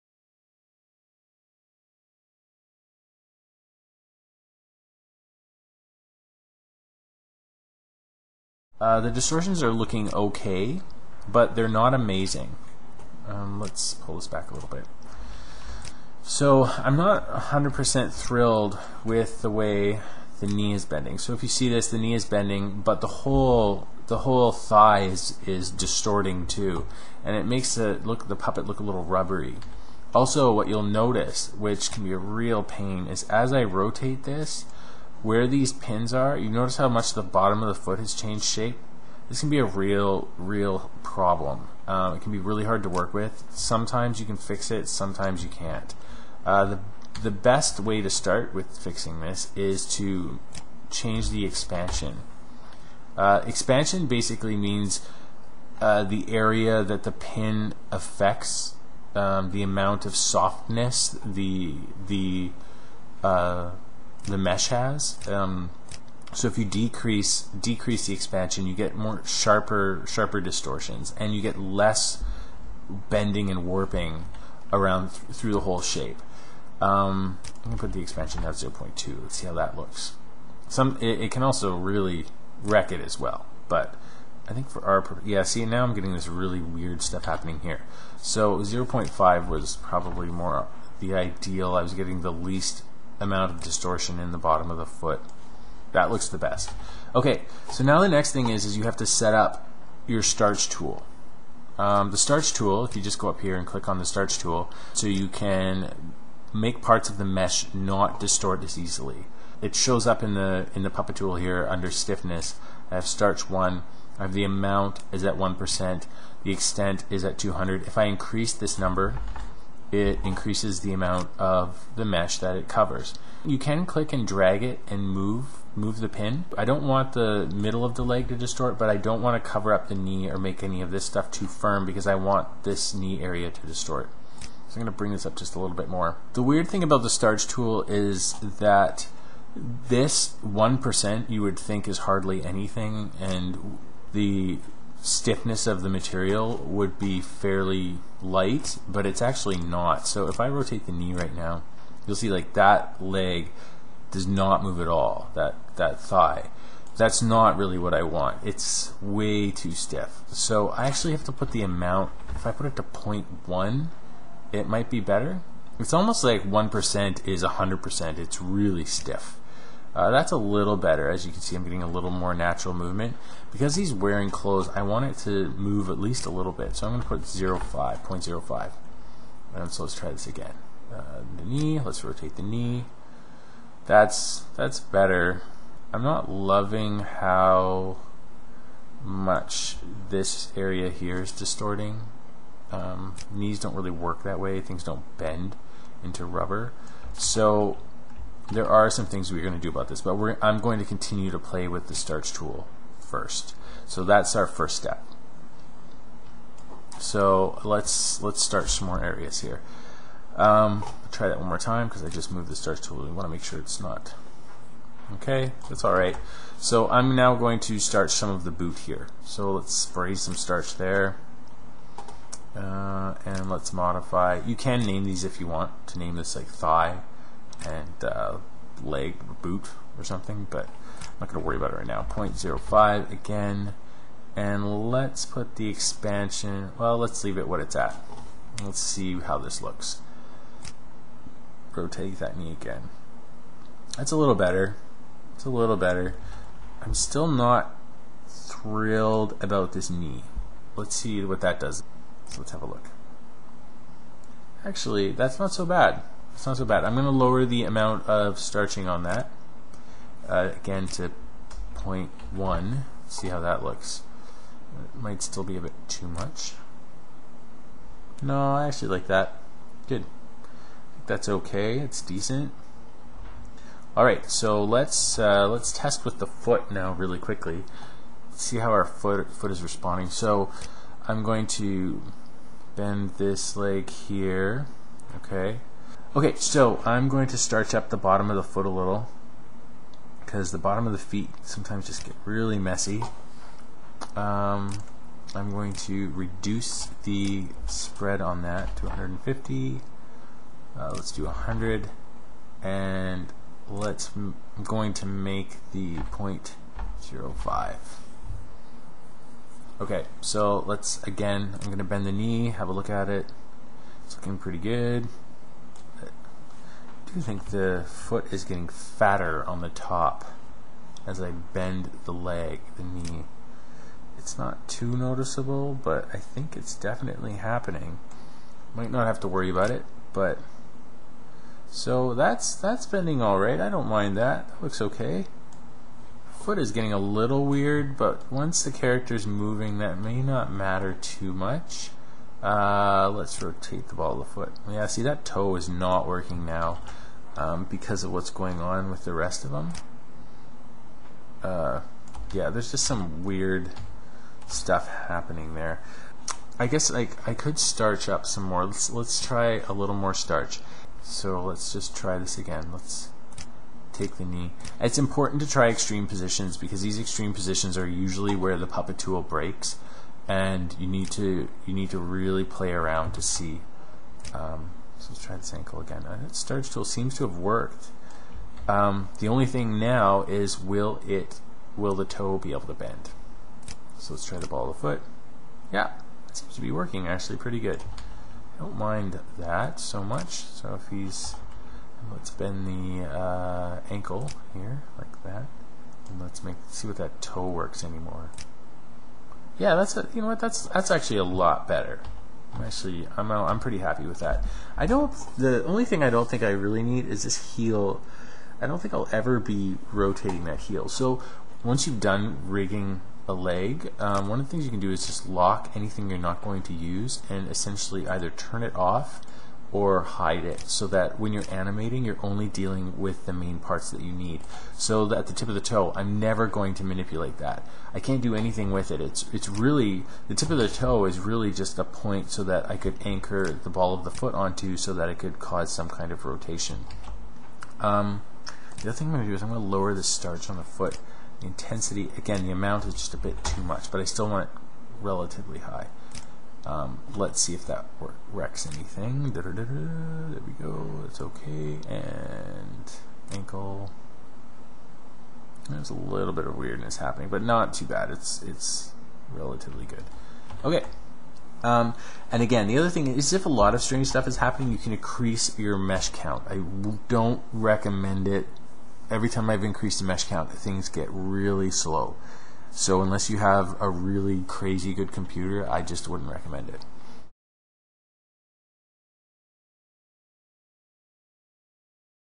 Lesson 2/4 | How To Rig a 2D Character Puppet in Adobe After Effects | Animation Tutorial
This Tutorial demonstrates how to refine the Puppet Tools mesh to make it bend and fold more realistically by utilizing the powerful Starch Tool.

Artwork provided by Ramon Perez for an upcoming independent animated short directed by Neil Christopher.